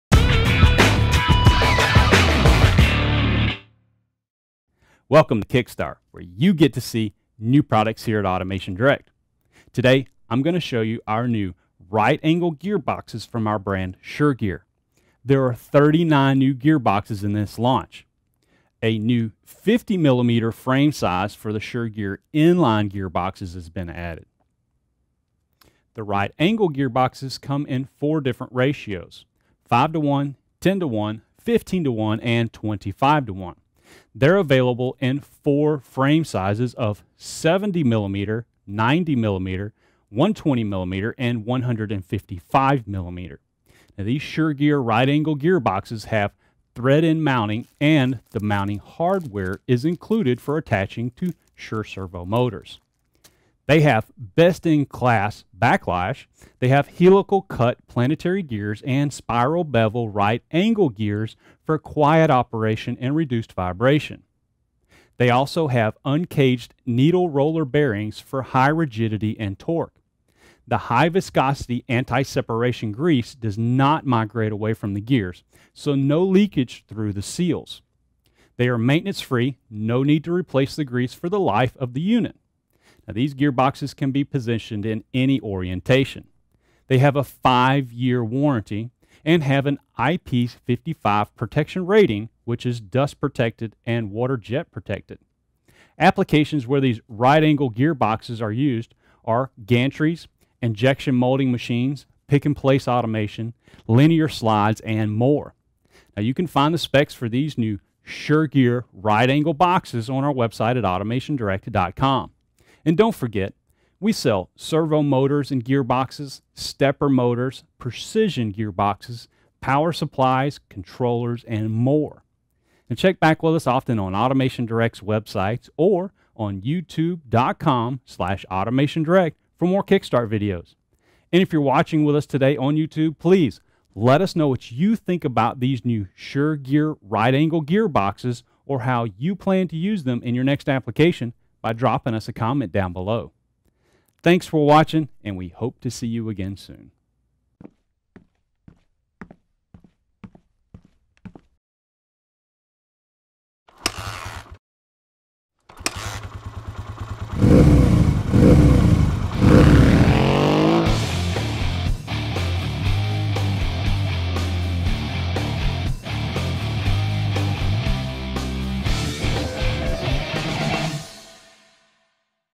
SureGear Right Angle Gear Box Overview from AutomationDirect
The SureGear PGB series of high-precision right-angle servo gear reducers is an excellent choice for applications that require a more compact footprint. The PGB right-angle planetary gear reducers offer similar technical specifications to the PGA series in-line gear reducers, and provides the customer with an excellent solution when space and clearance requirements are limited.